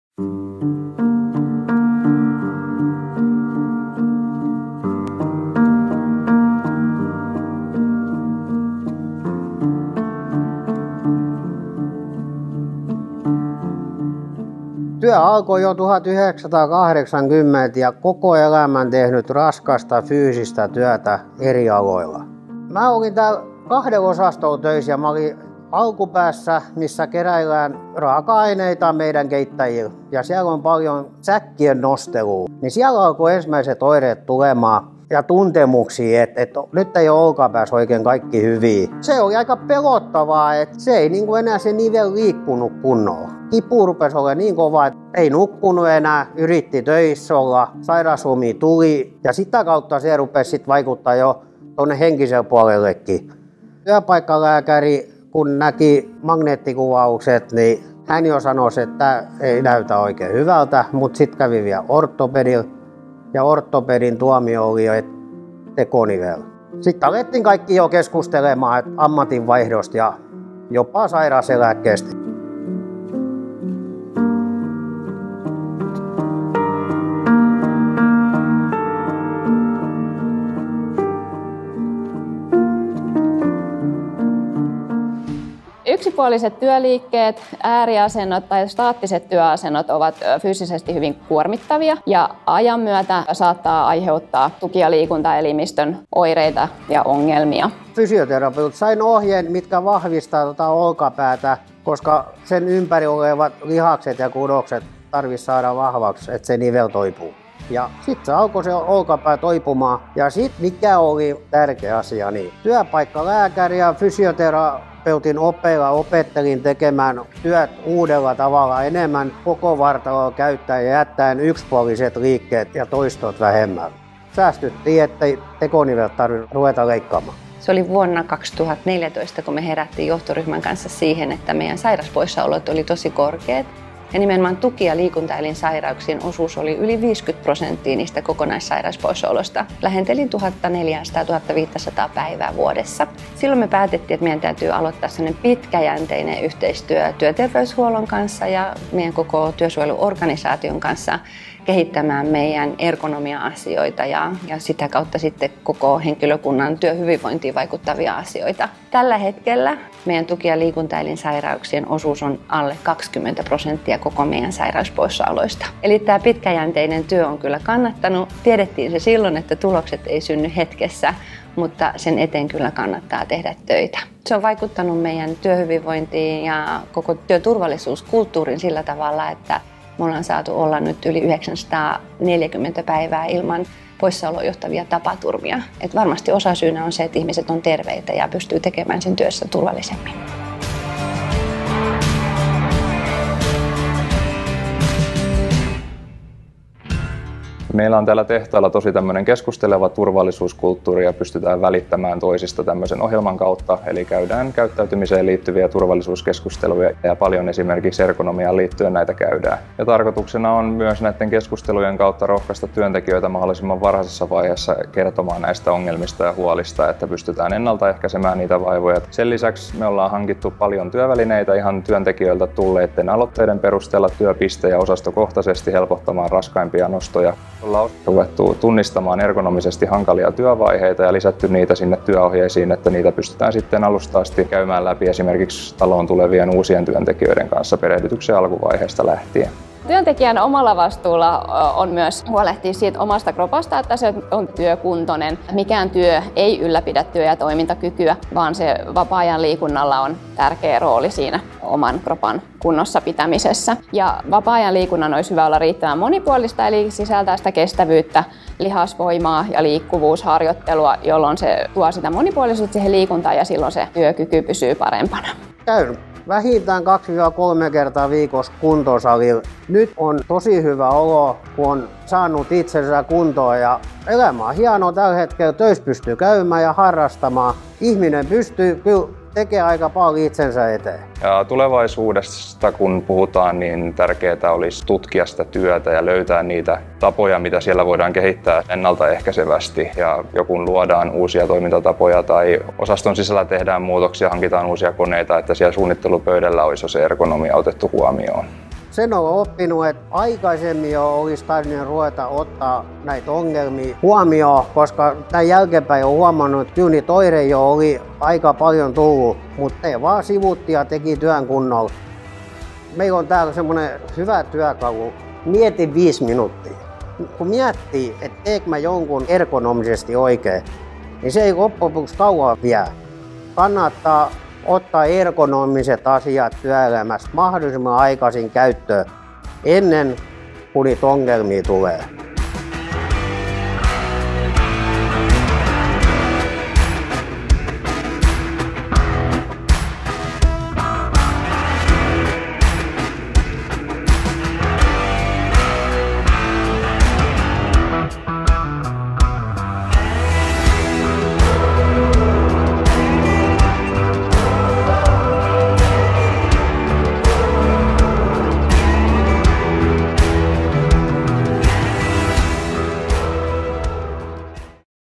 Ergonomics in prevention of musculoskeletal disorders at work
Pekka's story highlights how musculoskeletal disorders can be prevented at work and how investing in ergonomics is reflected in the company's safety culture.

This video has been produced by the Food Industry Branch Committee.